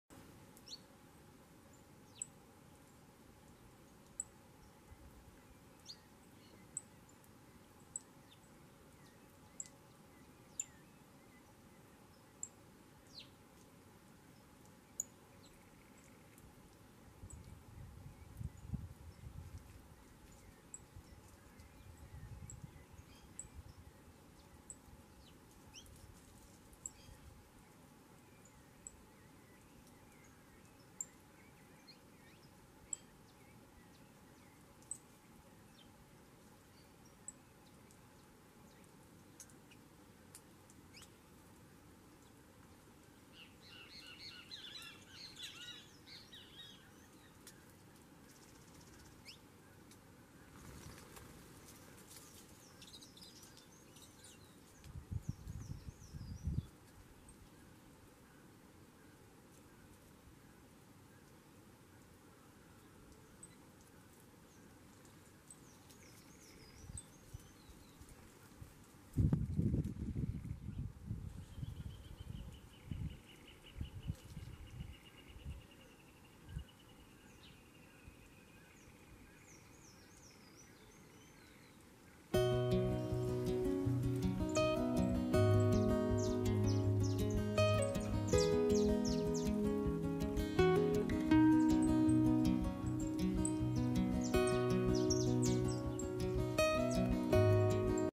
João de barro concertando a casa que o macaco quebrou
João de barro concertando a casa